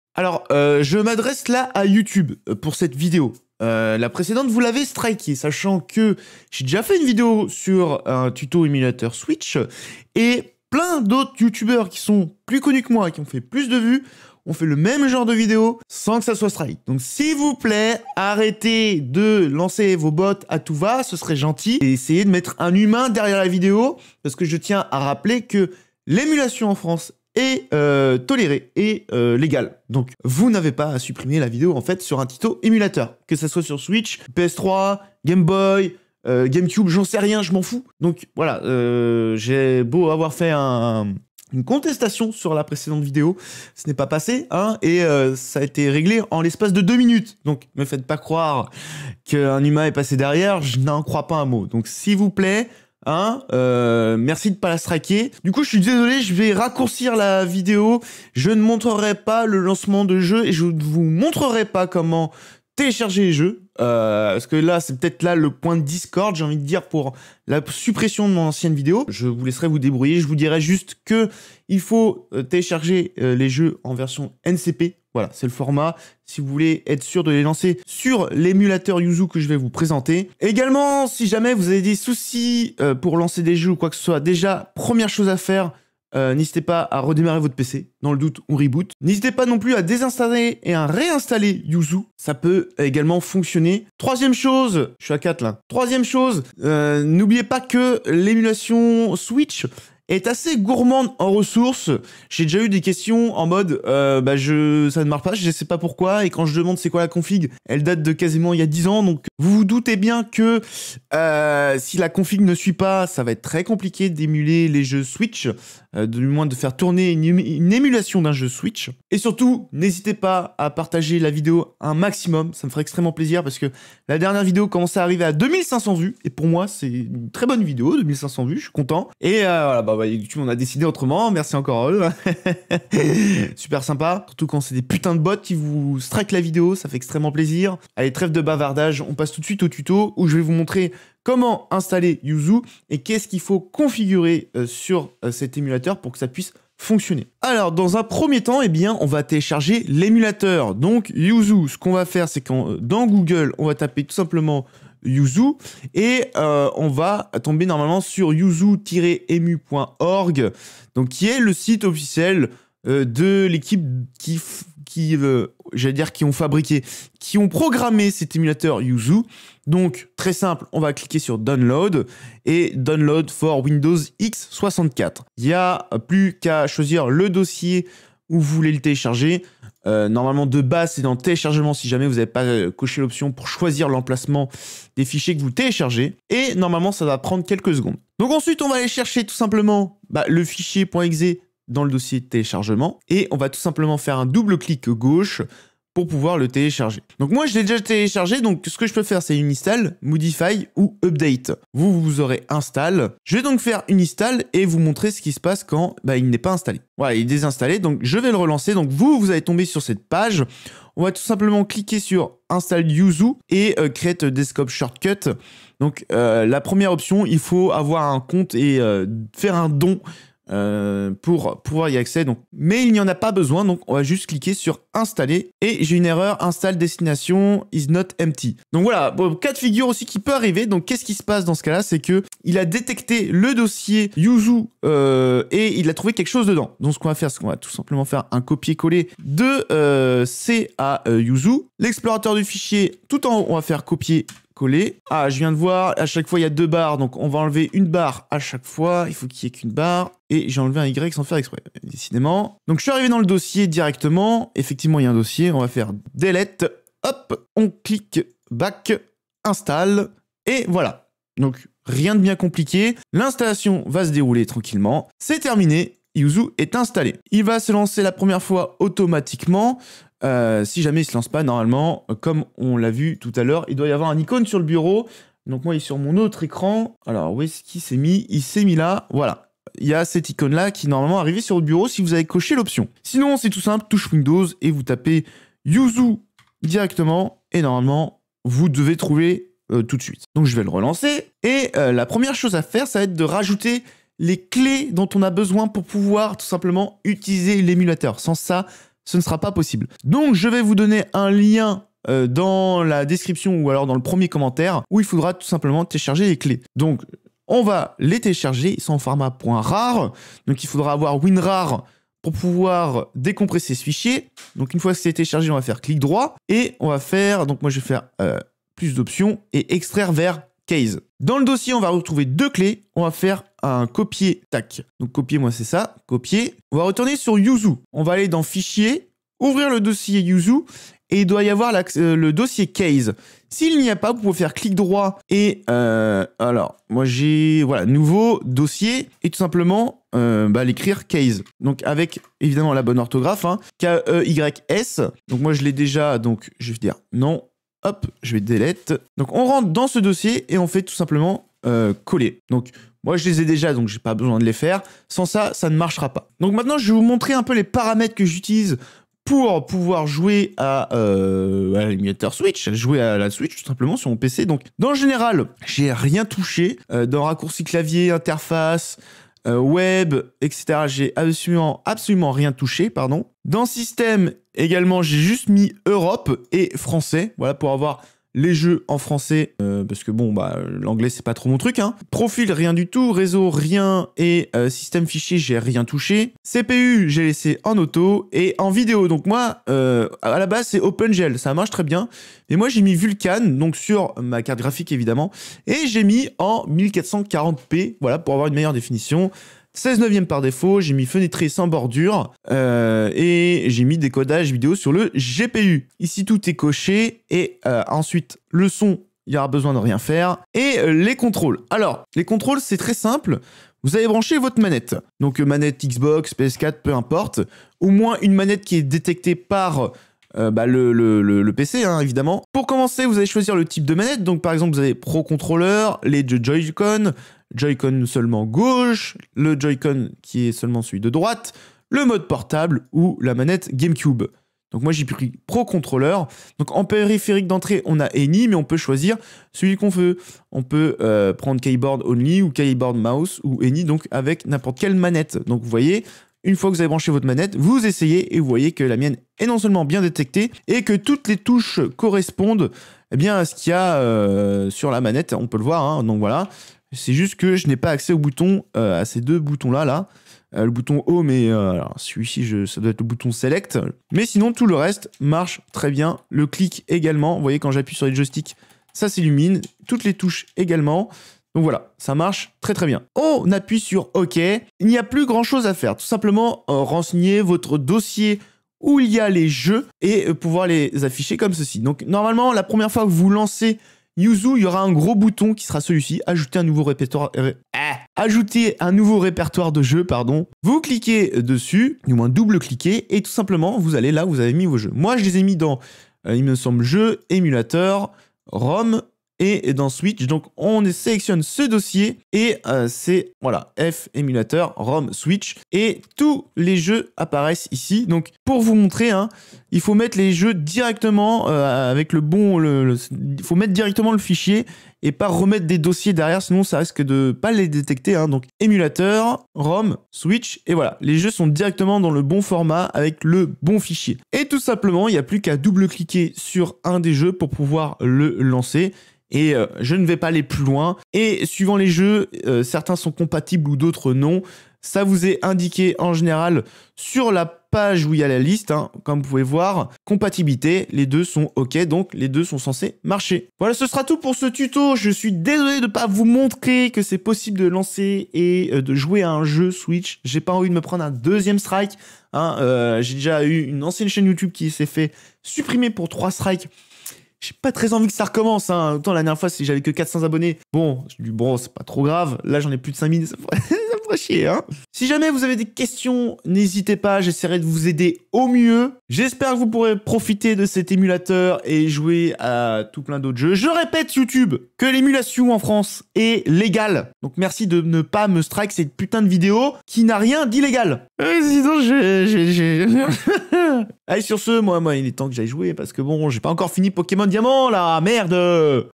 Tuto: émuler la Switch sur pc avec Yuzu ! [Tuto]
00:00 Introduction
01:09 Quelques infos
02:27 Installation Yuzu
05:00 Fichier Keys
05:56 Paramétrages
07:53 Liste de vos jeux
09:03 Outro

Mes objets en vente